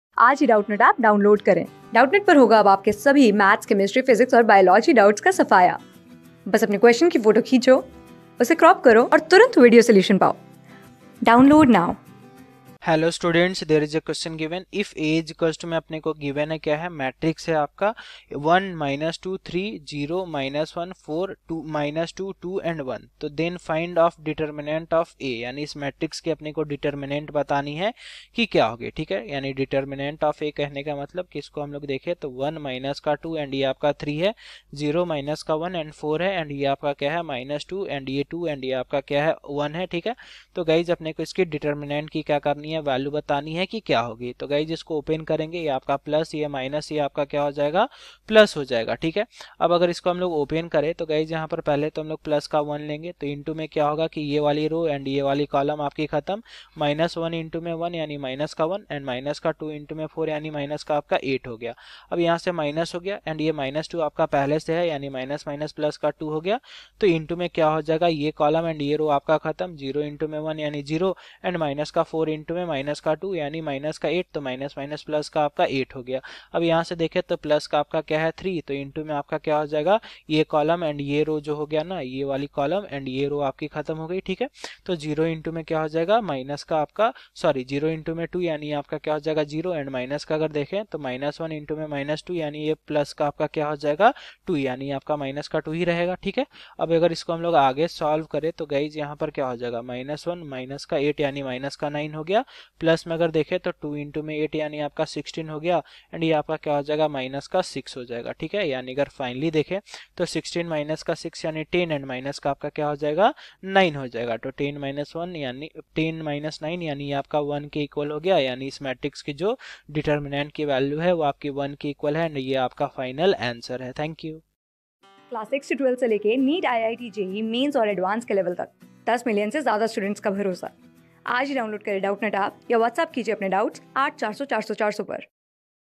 If A=[[1,-2,3],[0,-1,4],[-2,2,1]], then find | CLASS 11 | NONE | MATHS | Doubtnut
If A=[[1,-2,3],[0,-1,4],[-2,2,1]], then find |A|.
Class: 11
Subject: MATHS
Chapter: NONE
Board:USER_ASKED

👉 Have Any Query? Ask Us.